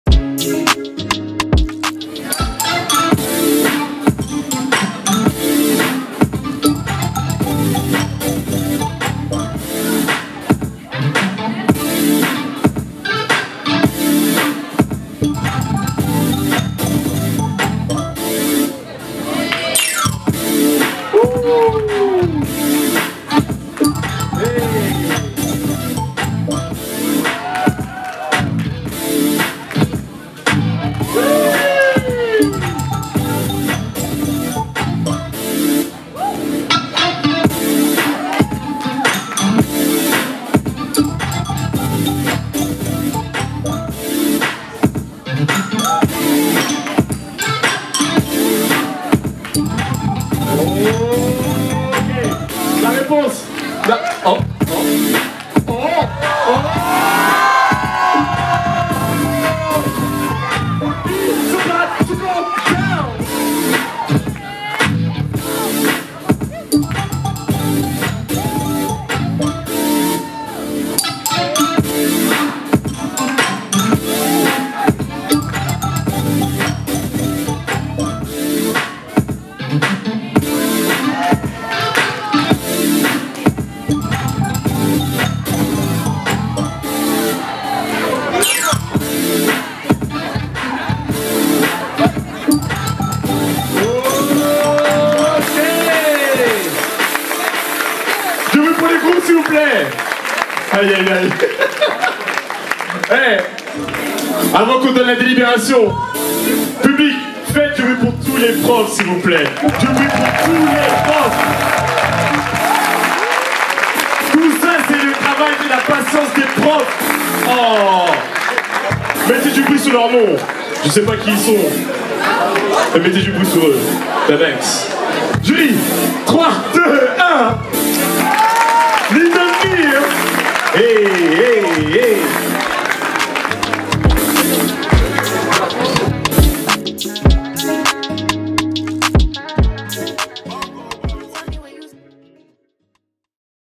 MAJESTIC vs LIGNE 2 MIRE - BATTLE 1vs1 - Y-INTERSCHOOL
Y-INTERSCHOOL 2021

Juges: JOYSON THIERNO MOUNIR
Speaker: JIMMY
Dj's: DJ SAM YUDAT / DJ MARKY MARK

Video : BARBE NOIR